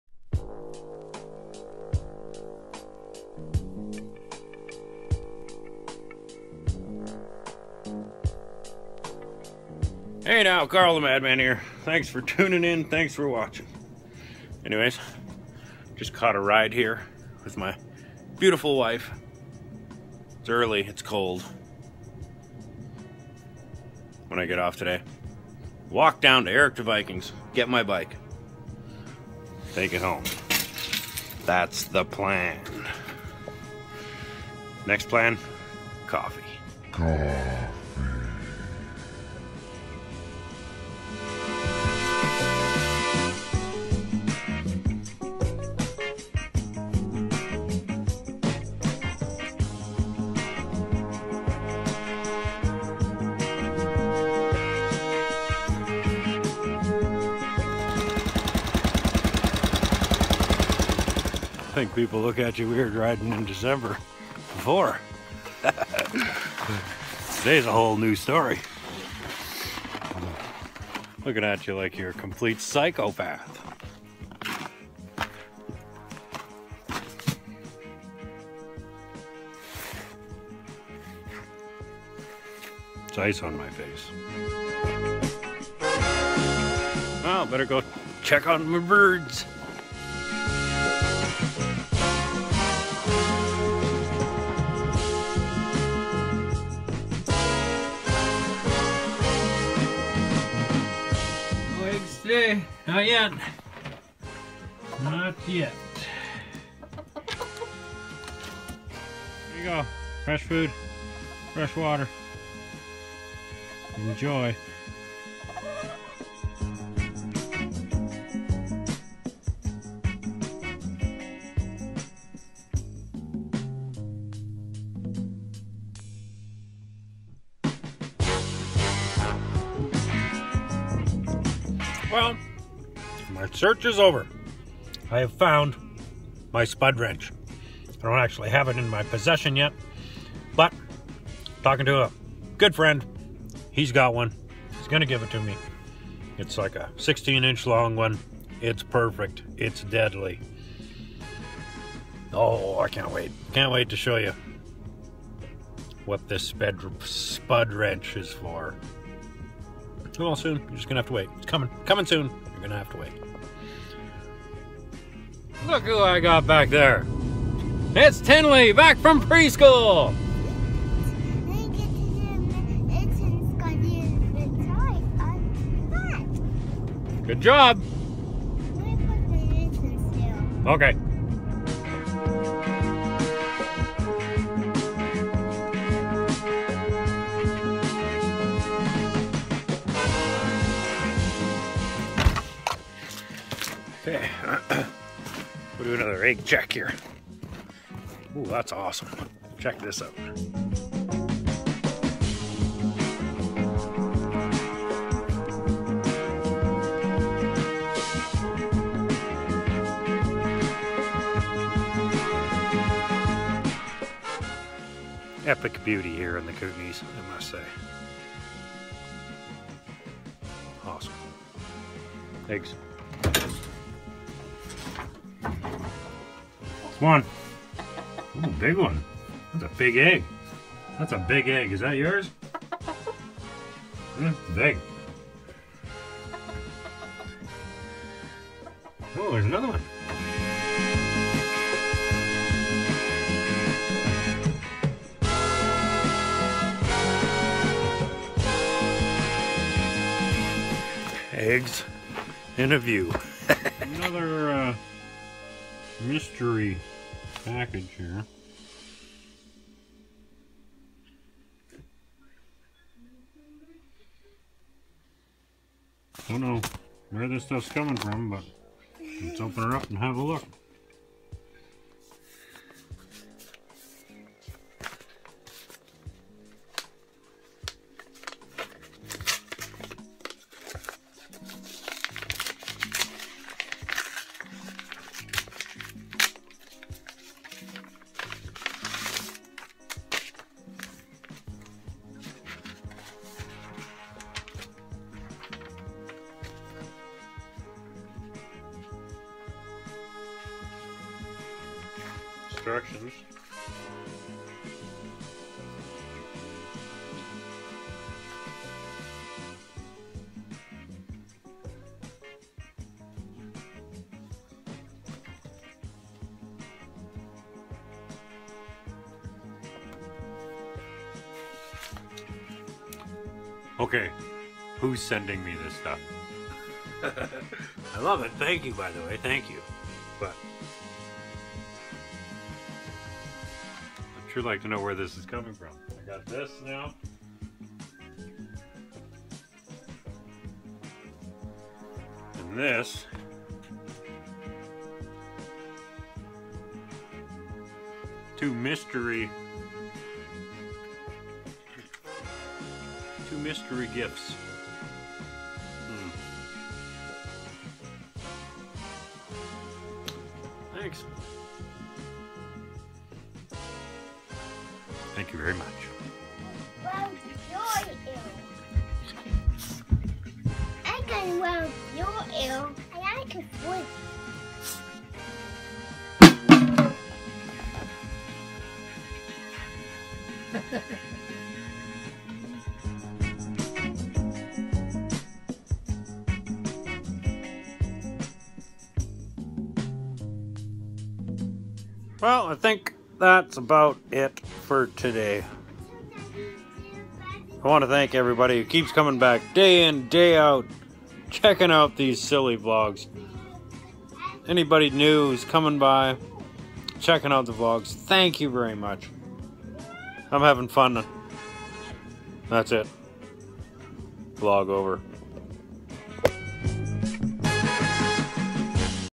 VLOG 105: "CHECK THAT OUT" 12\13\2016
Hey Now CarlTheMadman here!

It TODAYS episode of CTM TV you'll see coffee being drank and motorcycle in the snow. Did Chris the Chicken lay an egg??? TODAYS MORSE CODE! My search is over, another surprise in the mail and a thank you to the old and the new.